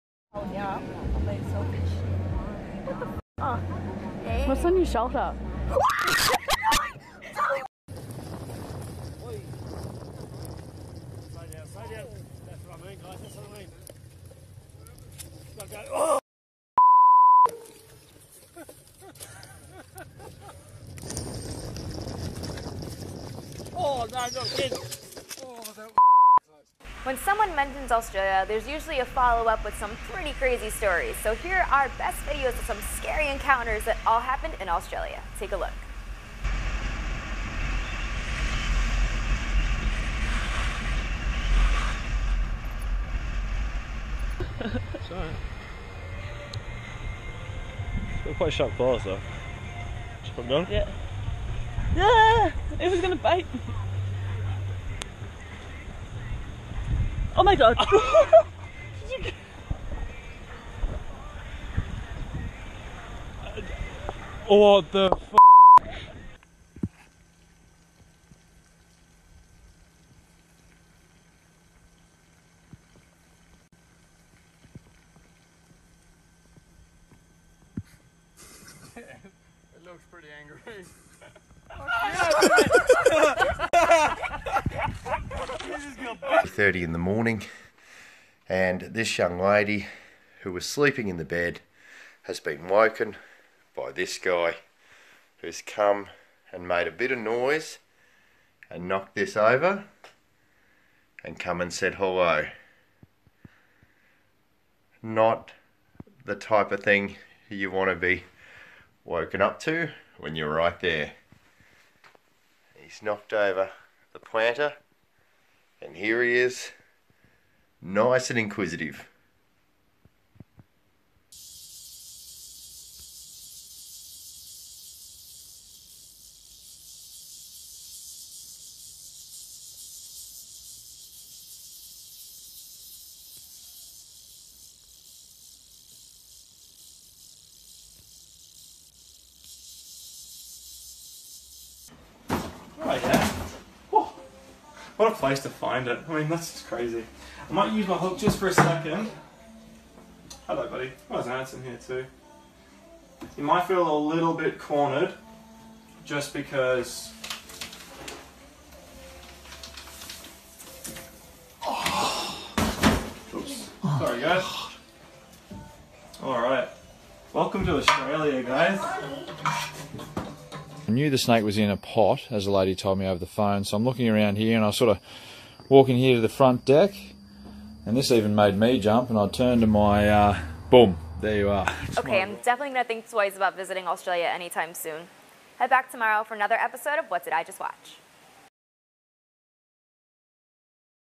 Funny Video Compilation: Australian Wildlife can be scary
Funny Video Compilation of Australian Wildlife that you wouldn't want to be confronted with! Funny AF